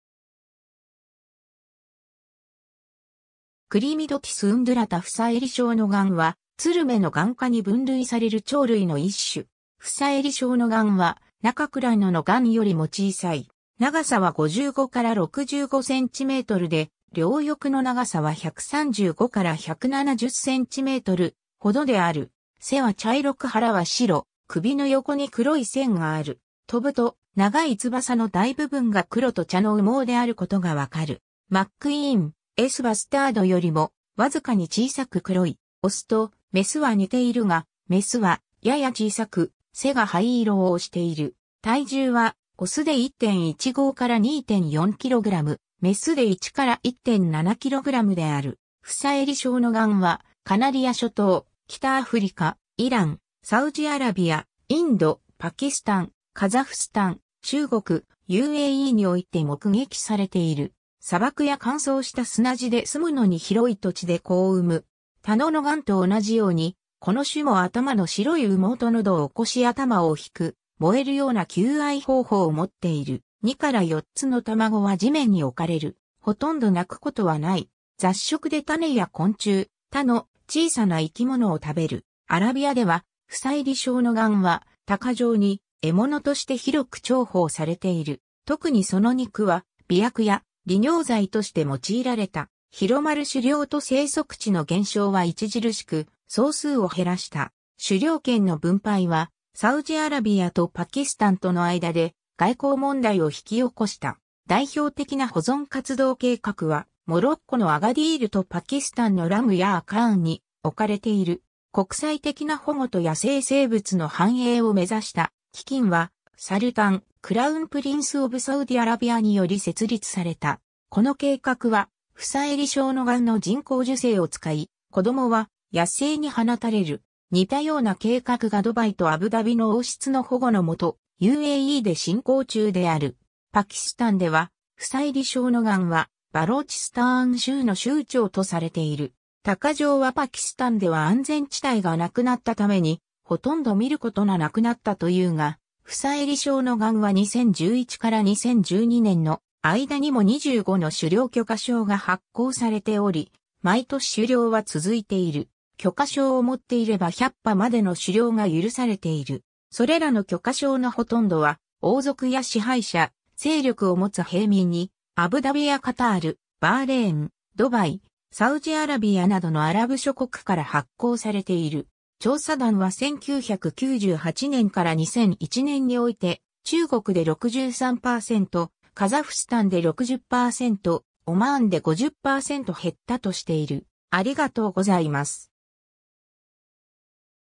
フサエリショウノガン
Chlamydotis undulata フサエリショウノガン（房襟小野雁、Chlamydotis undulata）は、ツル目ノガン科に分類される鳥類の一種。
 フサエリショウノガンは中くらいのノガンよりも小さい。
長さは55-65cmで、両翼の長さは135-170cmほどである。
背は茶色く腹は白、首の横に黒い線がある。
飛ぶと長い翼の大部分が黒と茶の羽毛であることが分かる。
Macqueen's Bustardよりもわずかに小さく黒い。
オスとメスは似ているが、メスはやや小さく背が灰色をしている。
体重はオスで1.15-2.4kg、メスで1-1.7kgである。
 フサエリショウノガンはカナリア諸島、北アフリカ、イラン、サウジアラビア、インド、パキスタン、カザフスタン、中国、UAEにおいて目撃されている。
砂漠や乾燥した砂地で済むのに広い土地で子を産む。
 他のノガンと同じように、この種も頭の白い羽毛と喉を起こし頭を引く燃えるような求愛方法を持っている。
2から4つの卵は地面に置かれる。
殆ど鳴くことはない。
 雑食で種や昆虫、他の小さな生き物を食べる。
 アラビアではフサエリショウノガンは鷹匠に獲物として広く重宝されている。
特にその肉は媚薬や利尿剤として用いられた。
広まる狩猟と生息地の減少は著しく総数を減らした。
狩猟権の分配はサウジアラビアとパキスタンとの間で外交問題を引き起こした。
 代表的な保存活動計画はモロッコのアガディールとパキスタンのRahim Yar Khanに置かれている。
国際的な保護と野生生物の繁栄を目指した基金はSultan, Crown Prince of Saudi Arabiaにより設立された。
この計画はフサエリショウノガンの人工授精を使い、子どもは野生に放たれる。
似たような計画が ドバイとアブダビの王室の保護のもとUAEで進行中である。
 パキスタンではフサエリショウノガンはバローチスターン州の州鳥とされている。
鷹匠はパキスタンでは安全地帯が無くなったために殆ど見ることな無くなったというが、フサエリショウノガンは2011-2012年の間にも25の狩猟許可証が発行されており、毎年狩猟は続いている。
許可証を持っていれば百羽までの狩猟が許されている。
それらの許可証のほとんどは王族や支配者、勢力を持つ平民にアブダビやカタール、バーレーン、ドバイ、サウジアラビアなどのアラブ諸国から発行されている。
 調査団は1998年から2001年において、中国で63%、カザフスタンで60%、オマーンで50%減ったとしている。